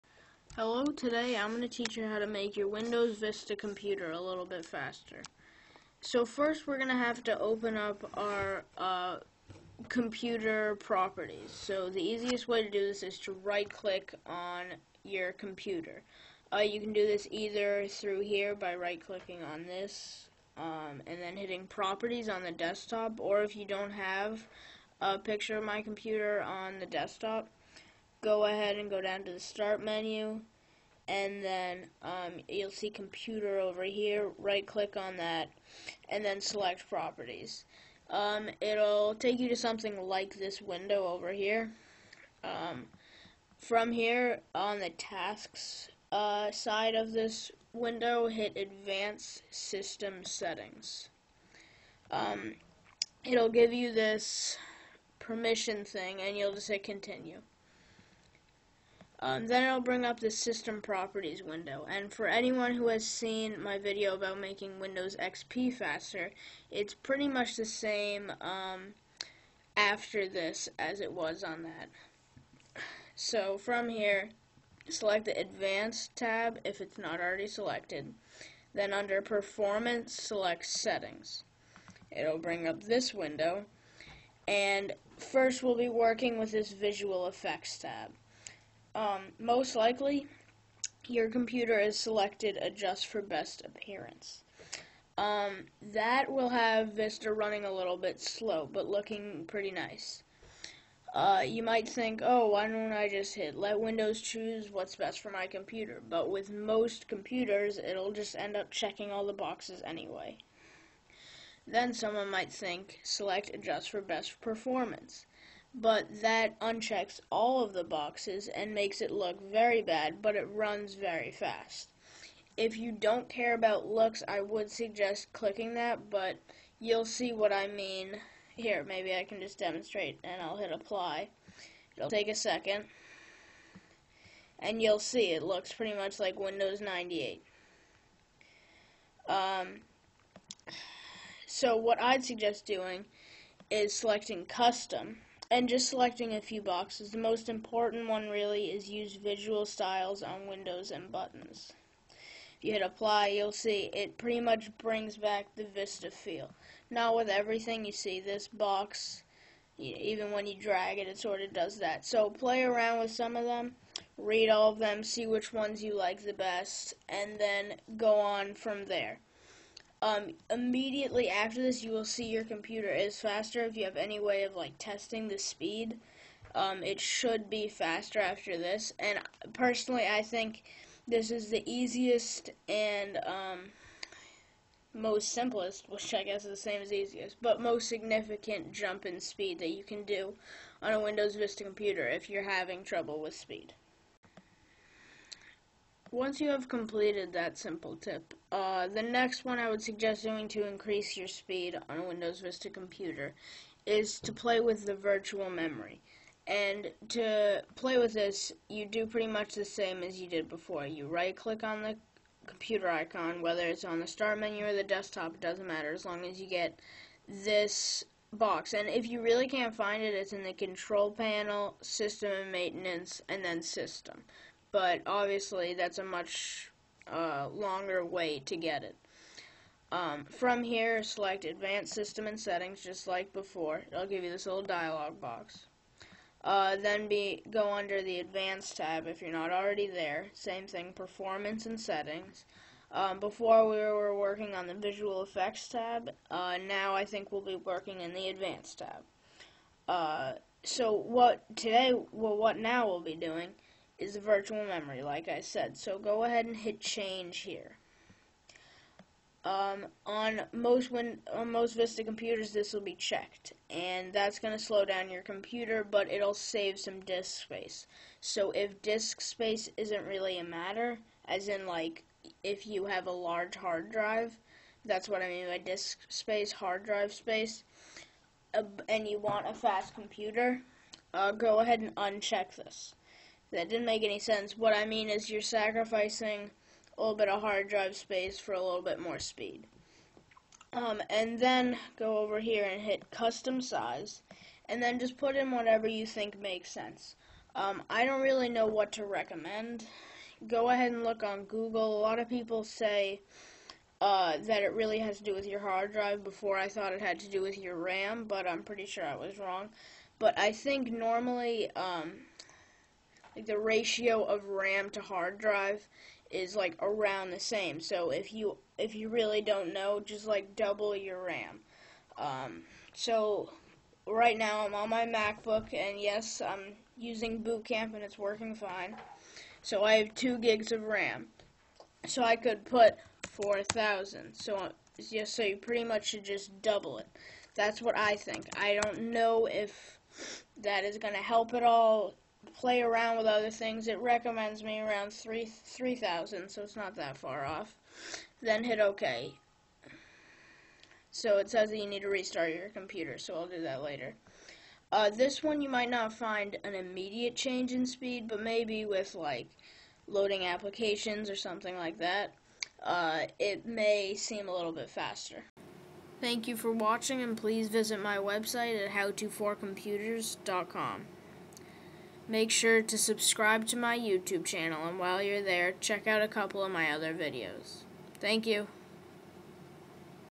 How To: Make Windows Vista Faster and More Efficient
Sorry about the colors, it was the screen recorder I used.
If you follow these simple steps, this should make your computer much faster. If you have any question, weather it is about this video or not, please ask it. I will answer most questions and maybe even make a video about it.